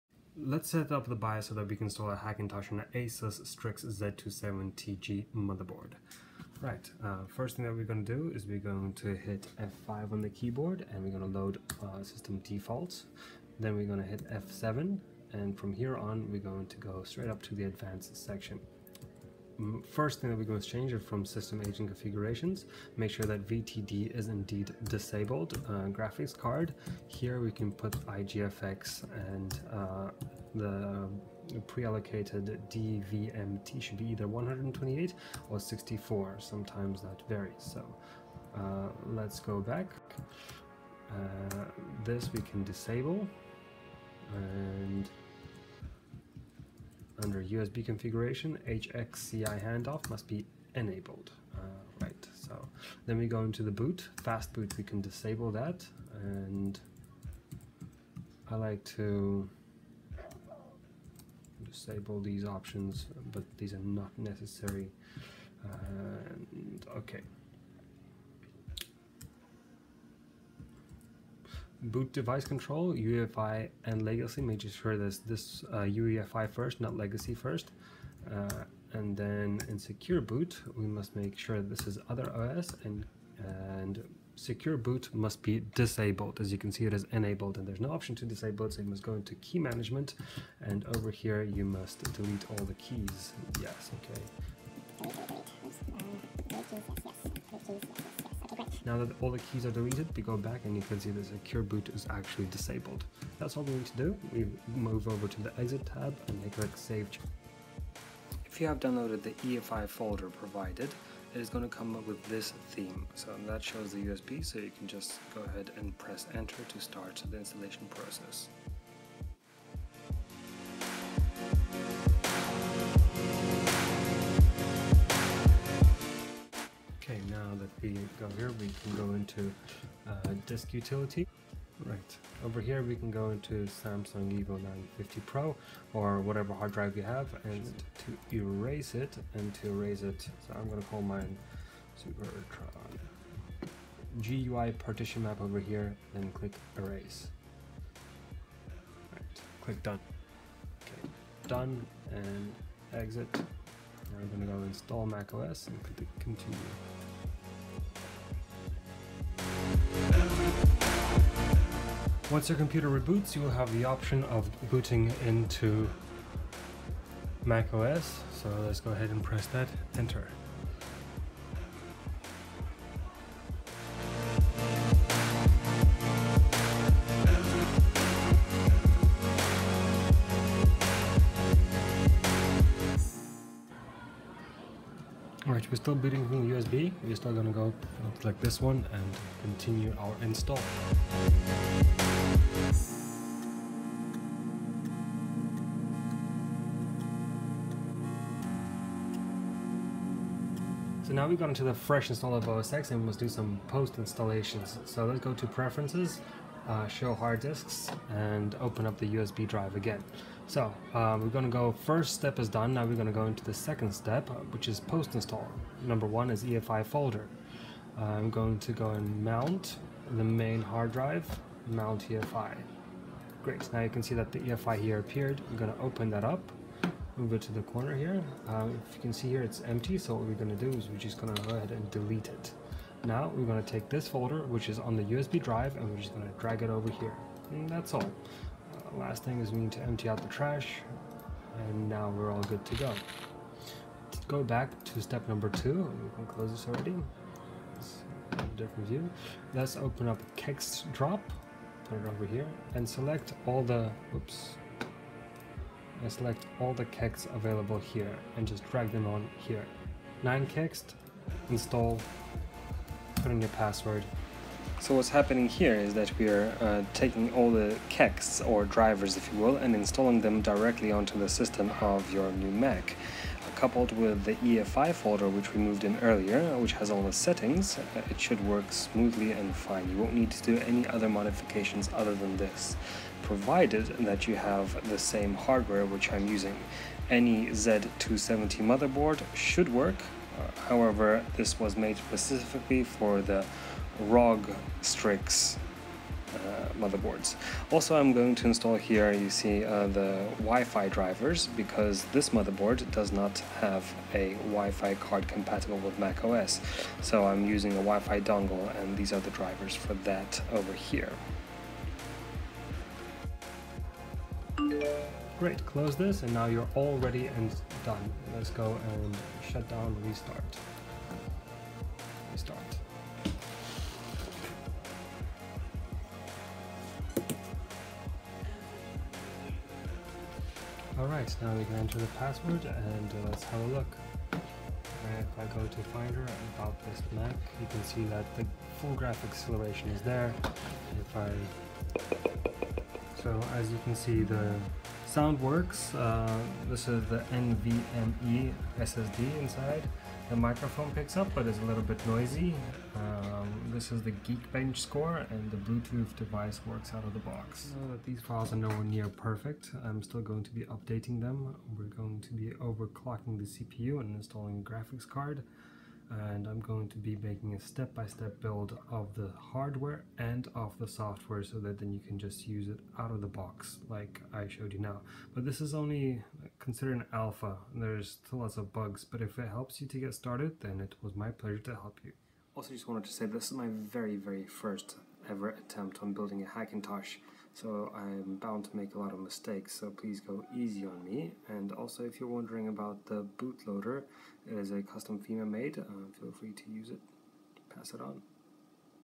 EASY Hackintosh on ASUS Z270G + Kabylake + NVMe + IntelHD630 *FILES INCLUDED*
4K Video Editing Hackintosh of 2017 (+Graphics Card)

This is a project aimed to make the best affordable Video Editing PC with Final Cut Pro X running Mac osX Sierra 10.12.5

Just download the files and and follow the video!

00:00 UEFI BIOS
01:45 Clover OSX install
03:20 Post Installation
05:32 Test and thought

You will need to make a Bootable USB with OSX and mont the EFI Drive and Drop in the Provided EFI folder - Before this video starts.

Specs:
ASUS ROG Strix Z270G Gaming - Motherboard
--- Intel HD630 - fully accelerated graphics
--- Bluetooth 4.0 - Working
--- Audio  -  Working
--- USB 3.0 - Working
--- WiFi on board - NOT Working!
--- Ethernet - Working
Intel i7 7700K KabyLake - CPU
Samsung EVO 960 250GB NVMe - m.2 SSD
HyperX 16GB 2400MHZ - RAM
NZXT Kraken x61 - Water Cooling
Corsair CX550M - PSU
Phantex Evol Entoo mATX - case
WiFi - USB Dongle

Known faults:
* Does not wake from sleep
- - - temp fix = System Pref / Energy Saver: Computer Sleep = Never
* Microphone sound is noisy